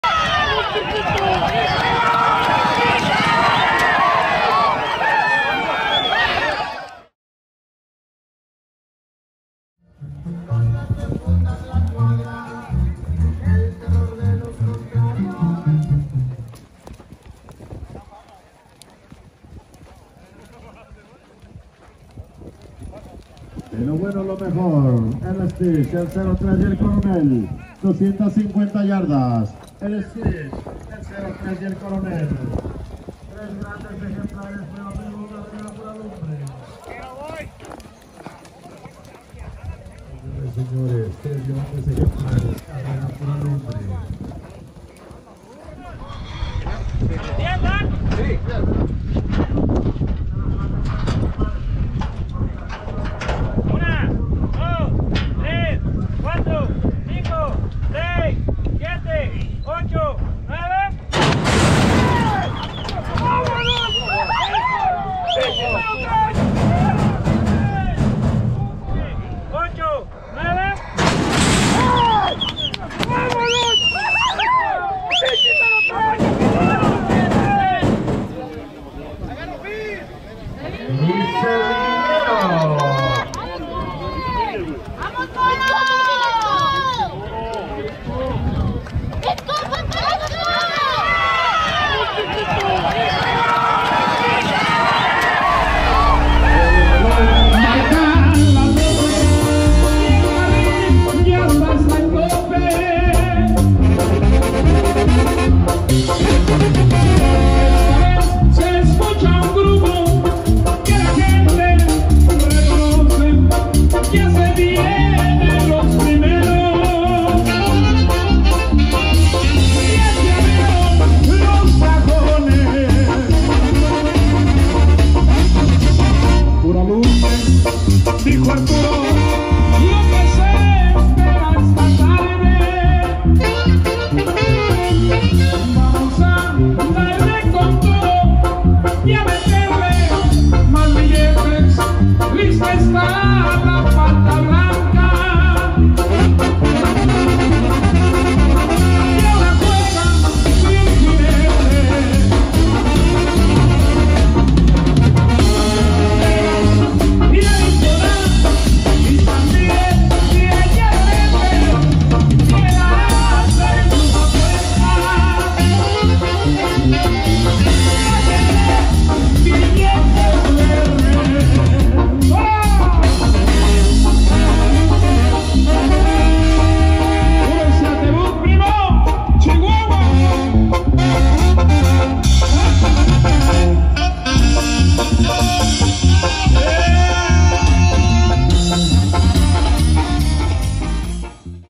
Stitch Cuadra Munvar vs El Cero Tres Cuadra Carrillo/DS vs El Coronel Cuadra Cometa
Stitch Cuadra Munvar vs El Cero Tres Cuadra Carrillo/DS vs El Coronel Cuadra Cometa en Carril Whiteglove 🔥🔥